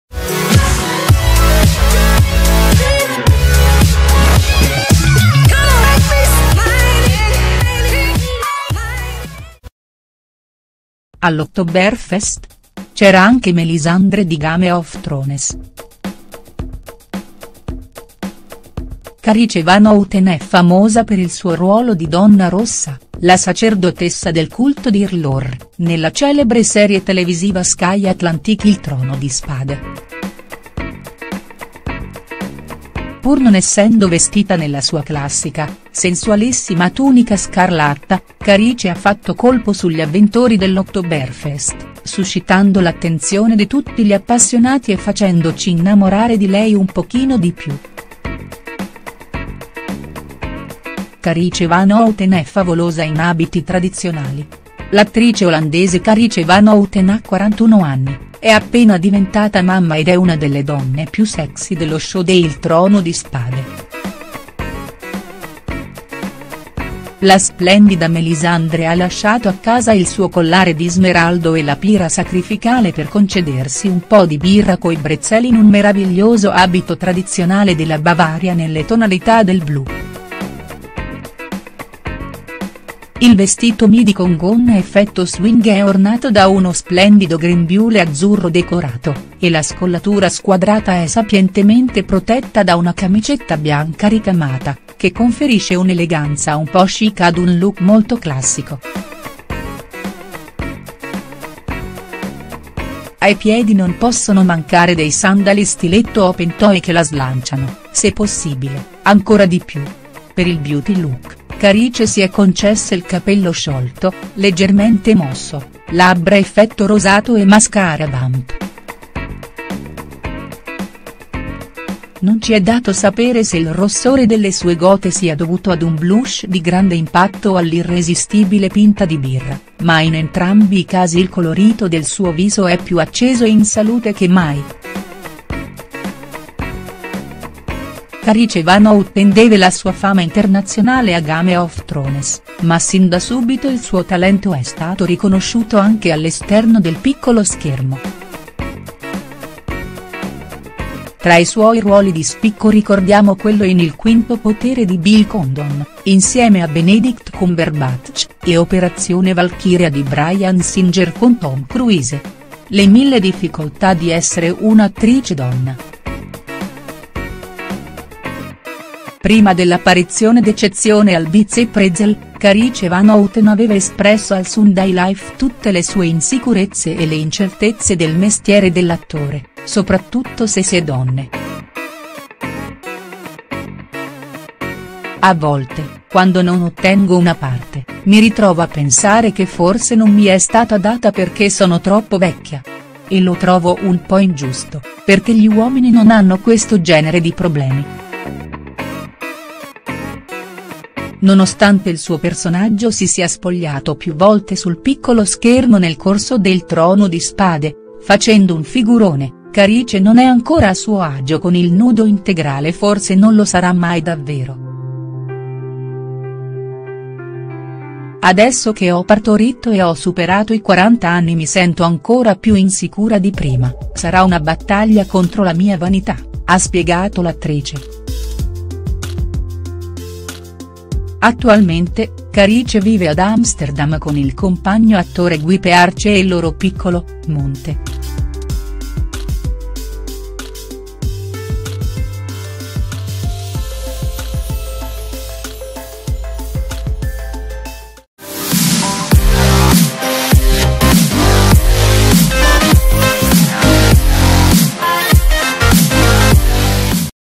All'Oktoberfest? C'era anche Melisandre di Game of Thrones.HD
Probabilmente più di qualche avventore dell’Oktoberfest si è strofinato gli occhi, incredulo, pensando di aver bevuto troppa birra. Invece Carice Van Houten, la sensuale Melisandre de Il Trono di Spade, era proprio lì, bella come un sogno, a godersi la magica atmosfera del festival bavarese della birra. E in abiti tradizionali!